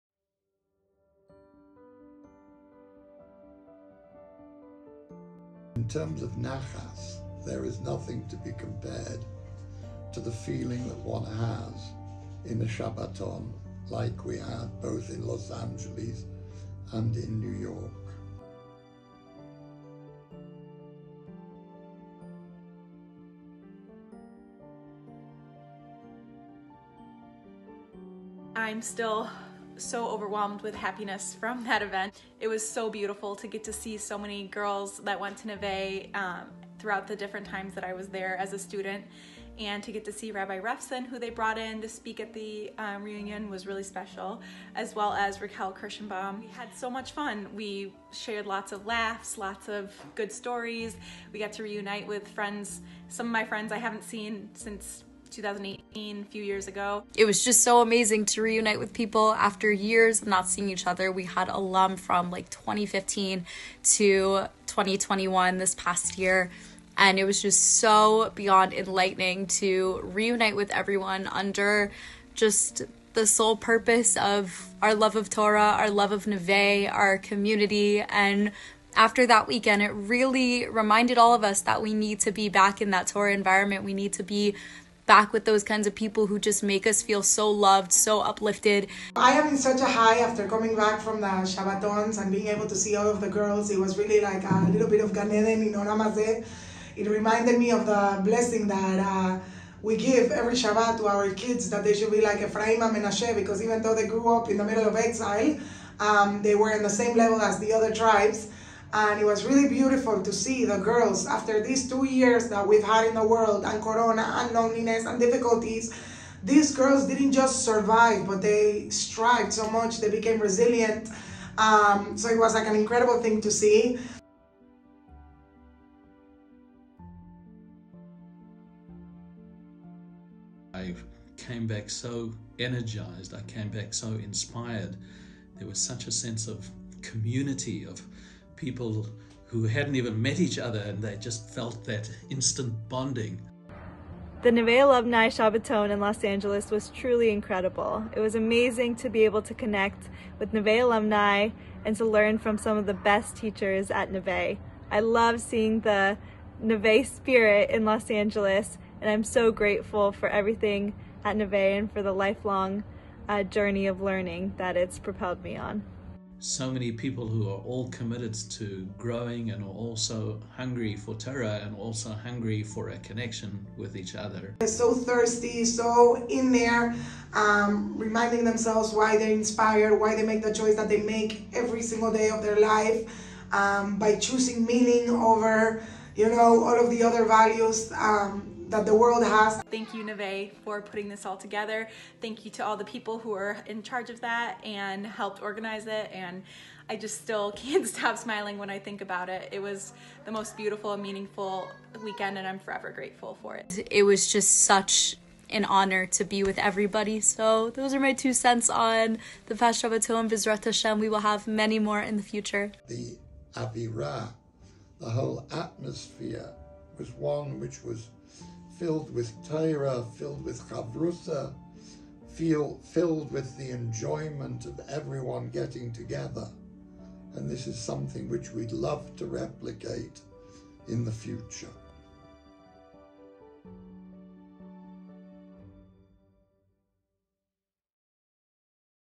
NeveShabbatons New York and Los Angeles 2021
Neve Alumnae reunite at a Shabbaton with Rabbi Dovid Refson, Rabbi Edelstein and Raquel